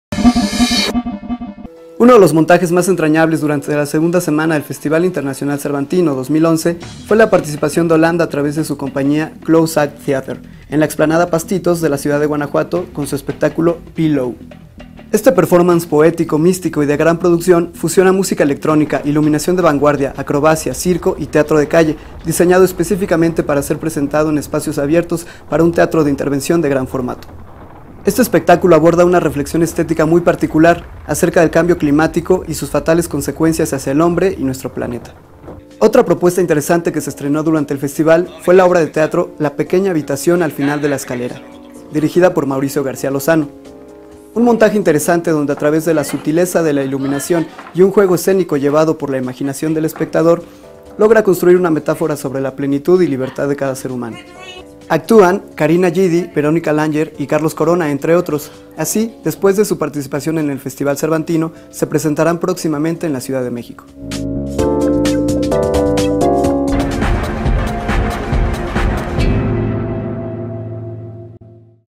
Dramafilia. Fantasía acústica llega al Cervantino
Uno de los montajes más entrañables durante la segunda semana del Festival Internacional Cervantino 2011, fue la participación de Holanda a través de su compañía Close-Act Theatre, en ña explanada "Pastitos" de la ciudad de Guanajuato, con su espectáculo PI-LEAU